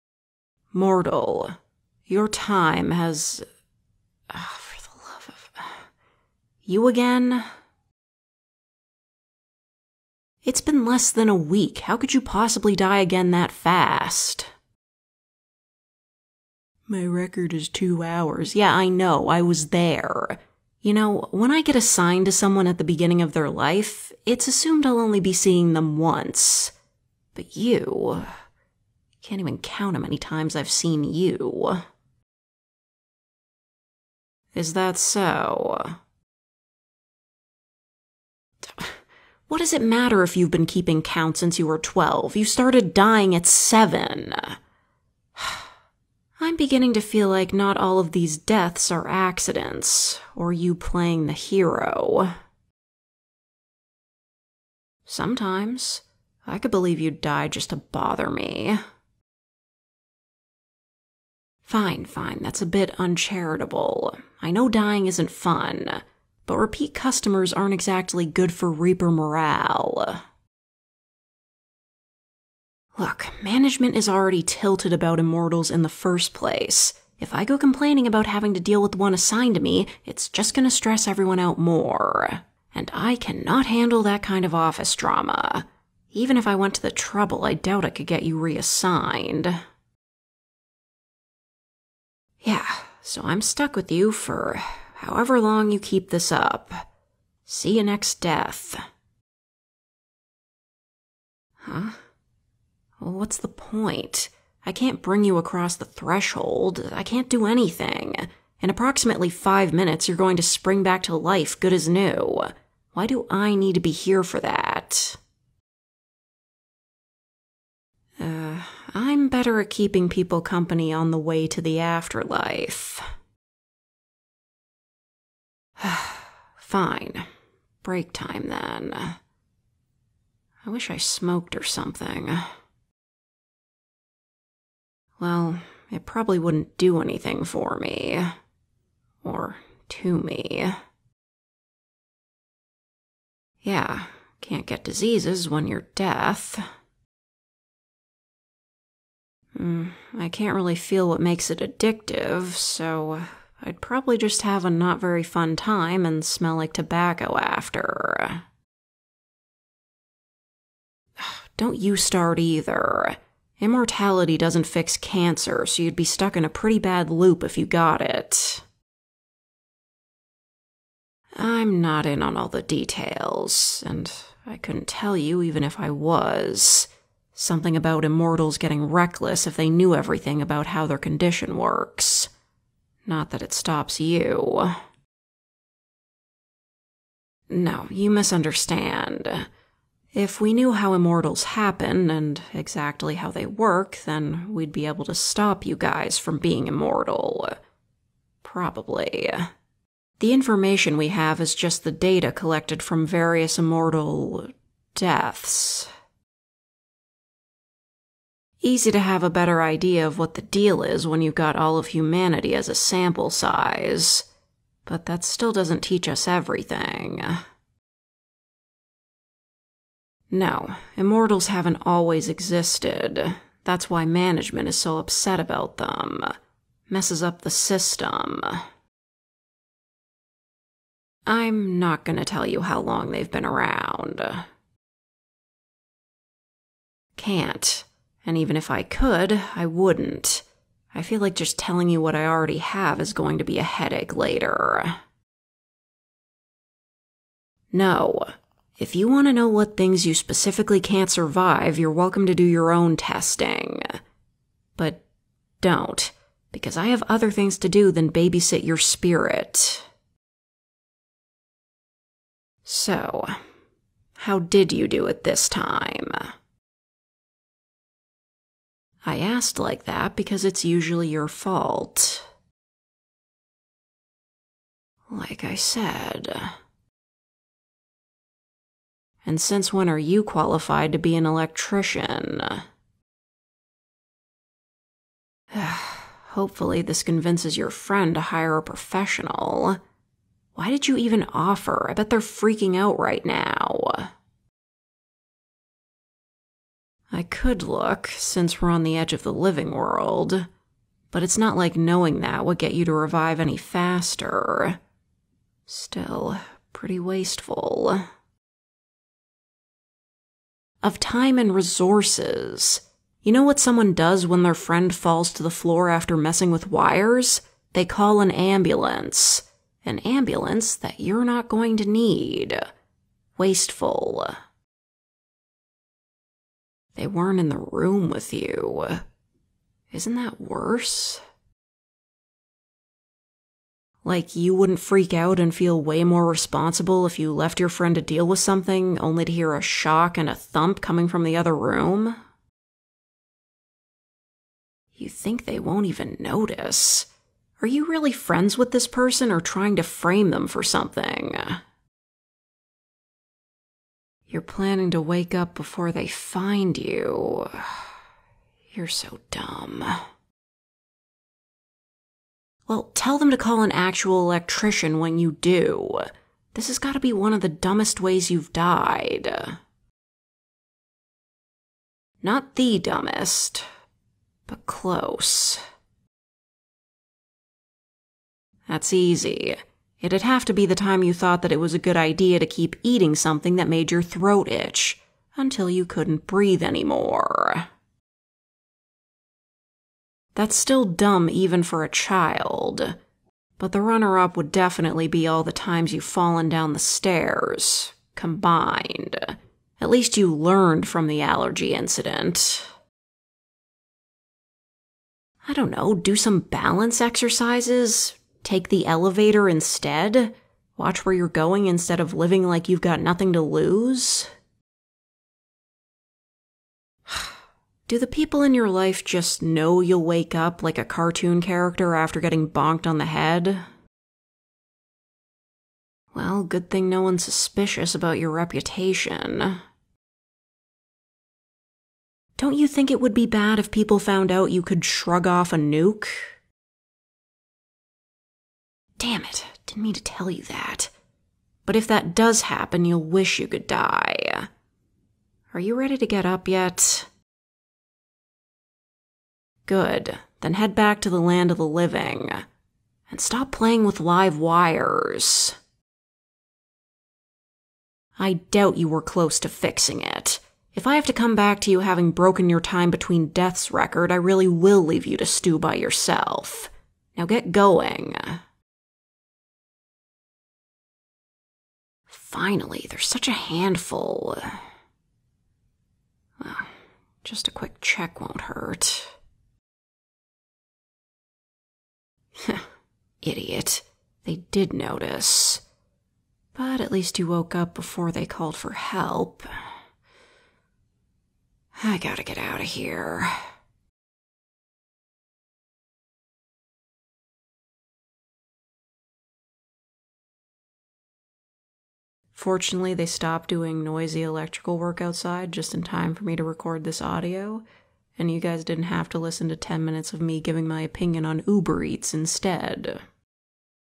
Encounter With A Grim Reaper Girl... Again (ASMR Roleplay) [F4A] [Immortal Listener]
Script: Keep living dudes
Listener: Neutral (Immortal)
Imagine getting shocked smh couldn't be me